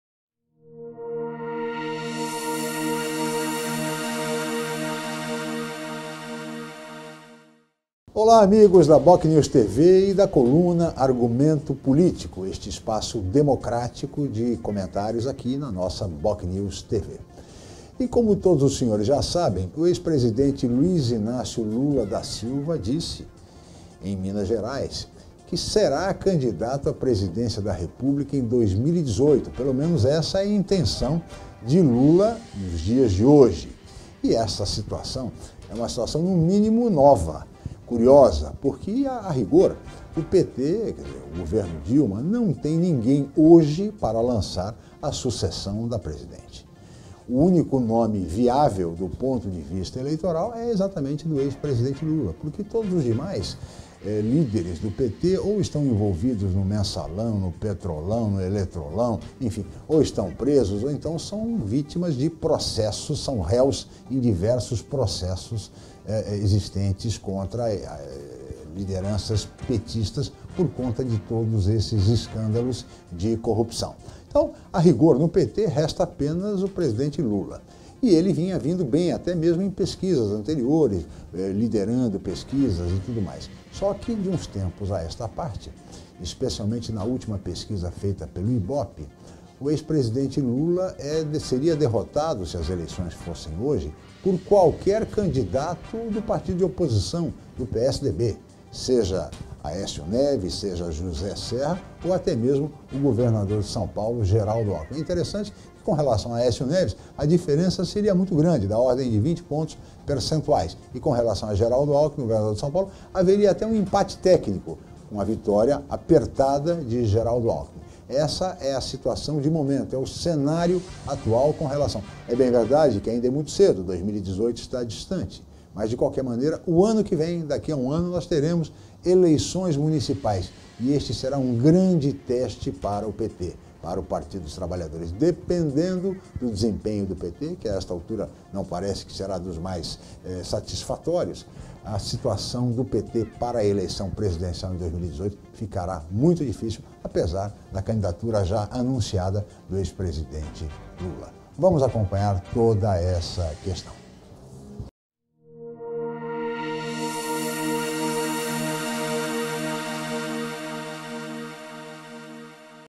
Lula admite disputar a Presidência em 2018
O jornalista Francisco La Scala Jr comenta a declaração do ex-presidente Luiz Inácio Lula da Silva que admite disputar a Presidência da República em 2018.